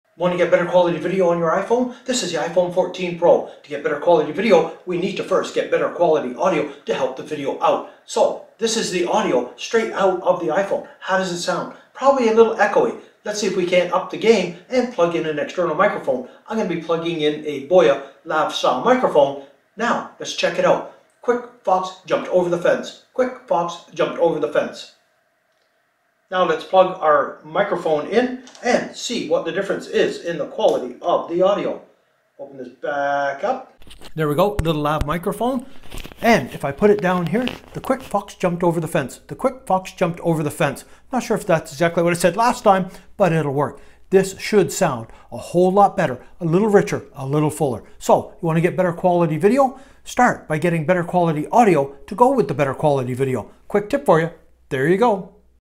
Iphone 14 pro how to get better quality video
Audio makes such a big difference to your videos. So not only do you need great video quality, you also need awesome audio!